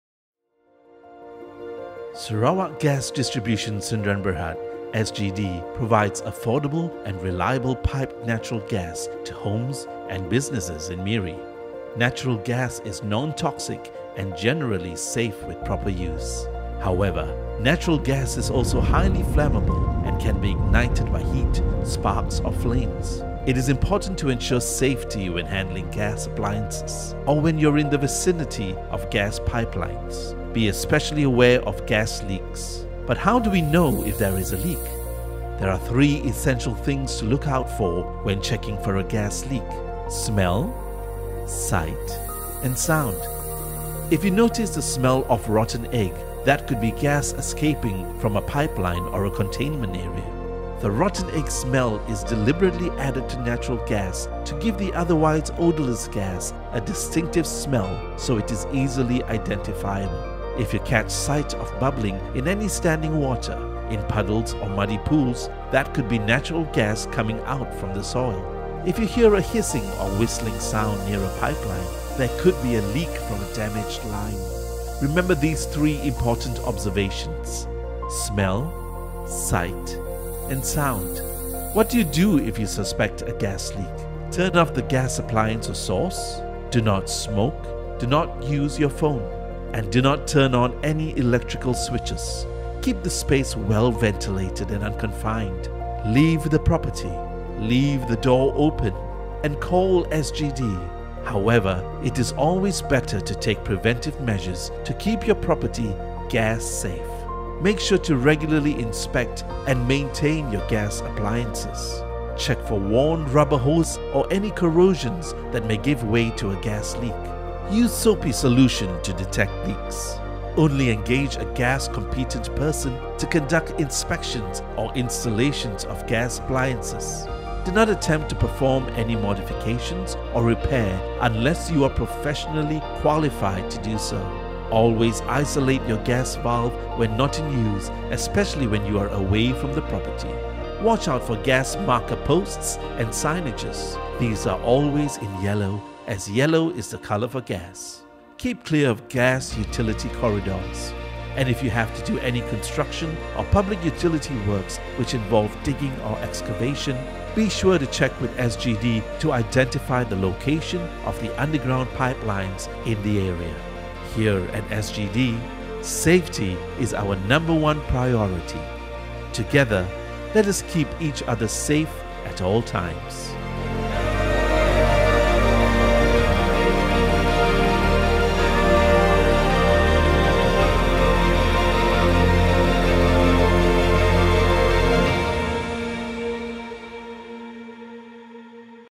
Natural Gas Safety Awareness Video
A Public Service Announcement on safety awareness for Natural Gas usage. CLIENT: PETROS | Sarawak Gas Distribution Sdn Bhd